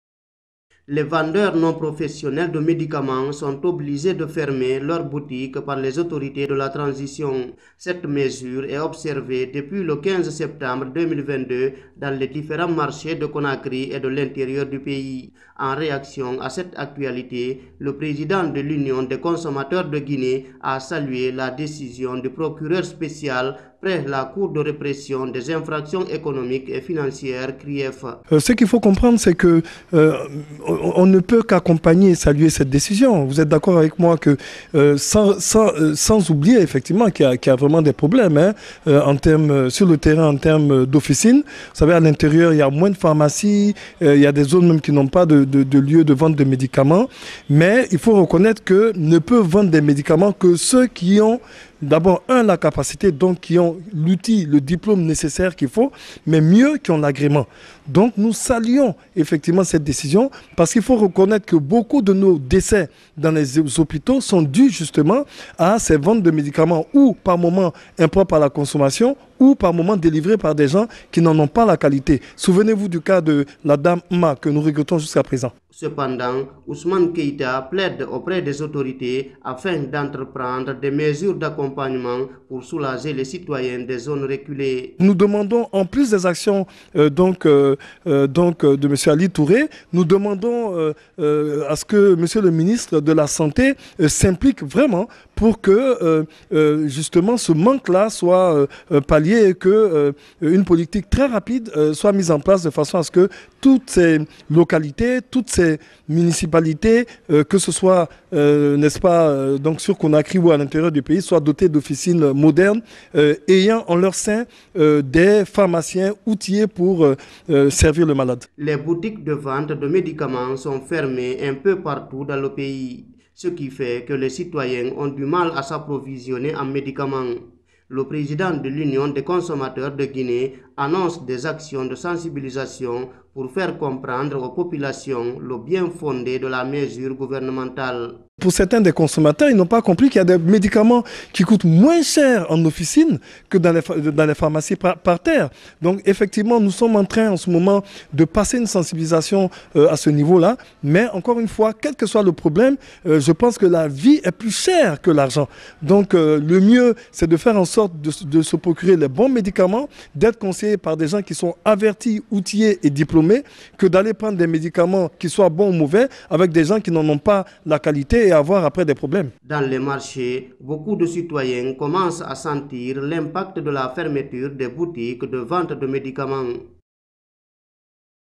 Fermeture des boutiques de vente de médicaments : Ousmane Keïta demande « des actions couplées »
La décision portant interdiction de la vente du médicament par les non-professionnels est entrée en vigueur depuis hier, jeudi 15 septembre 2022, en Guinée. Cette mesure vise à assainir le secteur du médicament dans le pays, en vue de préserver et protéger la santé de la population. Déjà, elle a conduit à la fermeture des points de vente illicite de médicaments à Conakry et dans les villes de l’intérieur du pays. Mais, cette fermeture, bien que salvatrice dans ce combat engagé par les autorités, pénalise de nombreux citoyens en quête de médicaments. Avec les prix exorbitants appliqués dans les pharmacies, ils sont nombreux les Guinéens qui n’arrivent pas à faire face à leurs ordonnances.

Dans un entretien accordé à Guineematin.com ce vendredi, 16 septembre 2022, Ousmane Keïta, le président de l’Union des consommateurs de Guinée, a salué cette interdiction imposée par les autorités. Mais, il a aussi plaidé en faveur d’actions couplées pour permettre au guinéen lambda d’avoir accès aux médicaments à un prix abordable.

« On ne peut qu'accompagner et saluer cette décision, sans oublier qu'il y a des problèmes en termes d'officines sur le terrain. Vous savez qu'à l'intérieur du pays il y a moins de pharmacies ; et, il y a des zones même qui n'ont pas de lieux de vente de médicaments. Mais, il faut reconnaître que ne peuvent vendre de médicaments que ceux qui ont capacité et l'outil, dont le diplôme ; et, mieux, ceux qui ont l'agrément. Donc, nous saluons cette décision, par ce qu'il faut reconnaître que beaucoup de nos décès dans les hôpitaux sont dus à la vente de ces médicaments impropres à la consommation ou délivrés par des gens qui n'en ont pas la qualité. Souvenez-vous du cas de la dame M'Mah Sylla que nous regrettons jusqu'à présent. On demande aux autorités de faire des actions couplées… Il faut reconnaître qu'à l'intérieur du pays, il y a un manque criard d'officines pouvant faire en sorte que les malades puissent aller se procurer des médicaments pour se soigner. Donc, nous demandons en plus des actions du procureur spécial de la CRIEF, que le ministre de la santé s'implique pour que ce manque là soit palier et qu’une politique très rapide soit mise en place de façon à ce que toutes ces localités soient dotées d'officines modernes ayant en leur sein des pharmaciens outillés pour servir les malades », a indiqué Ousmane Keïta.